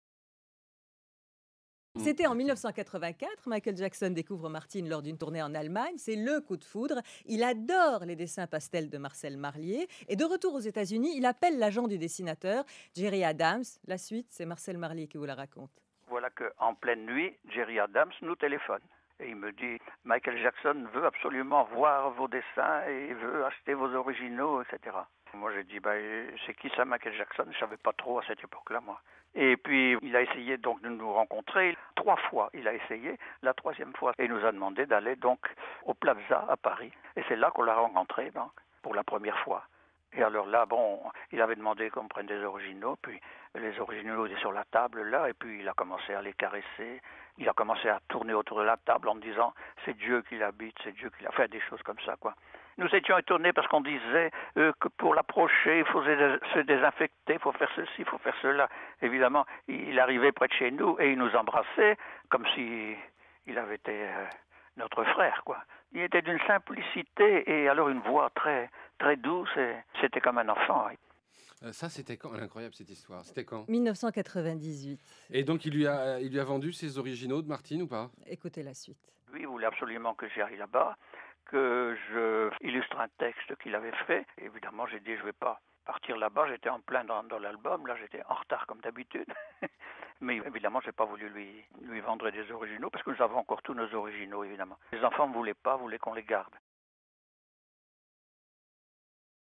Extrait archives M6 Video Bank // Interview de Marcel Marlier (RTL - 1998)
Extrait d'une interview de Marcel Marlier en 1998 diffusé dans une émission de 2009 sur RTL. Il se confiait sur la volonté de Michael Jackson d'acheter ses originaux de Martine.

Toutes nos archives RTL sont disponibles à la vente pour les professionnels de l'audiovisuel et des médias